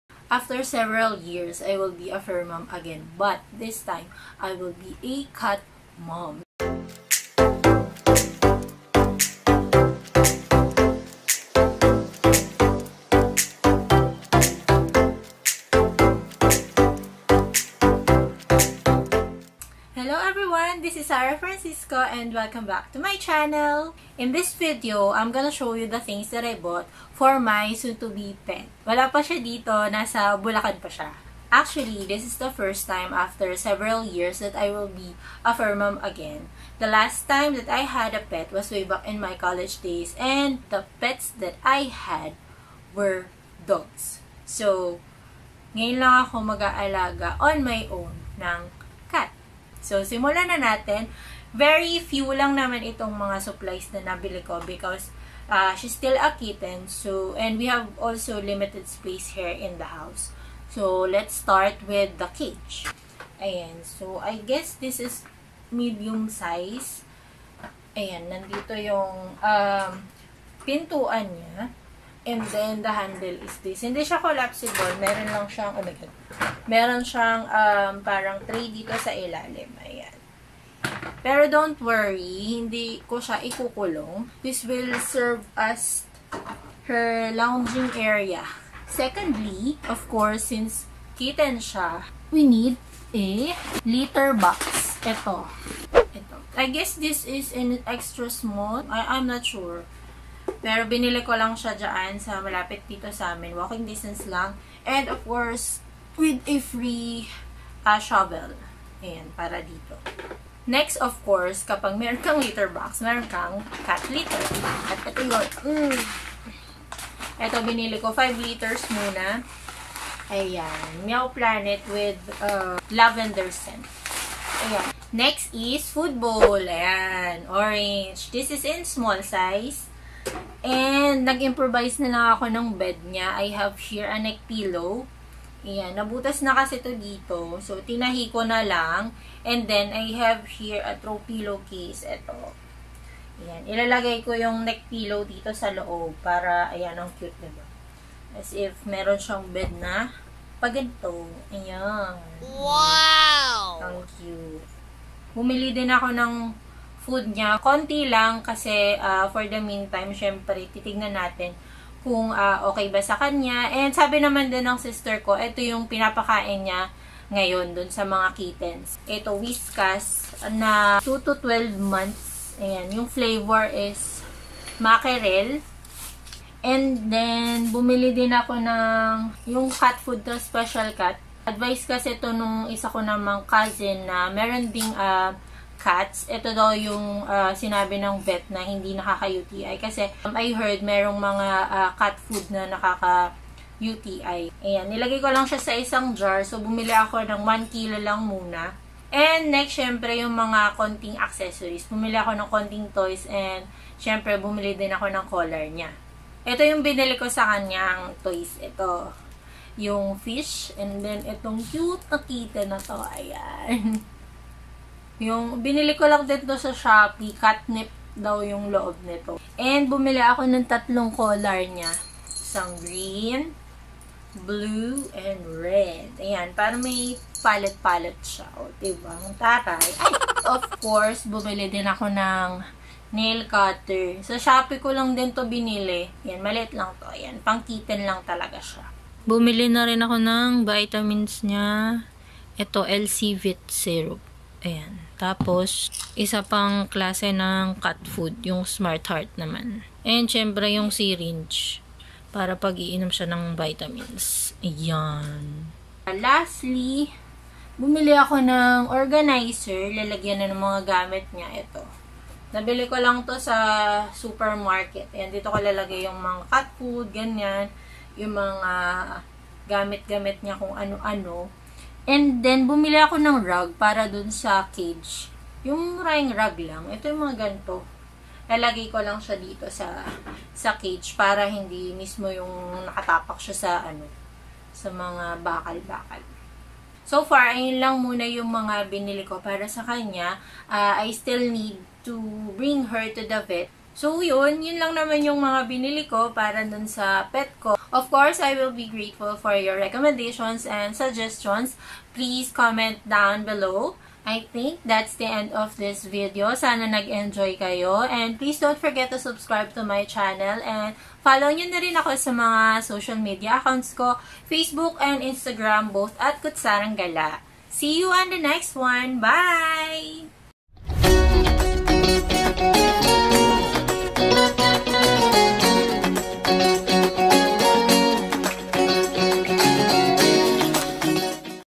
PET SUPPLIES HAUL / BASIC CAT SUPPLIES - FIRST TIME FUR MOM! | Sarah Francisco
This is my first time to be a cat mom. I hope I can do this!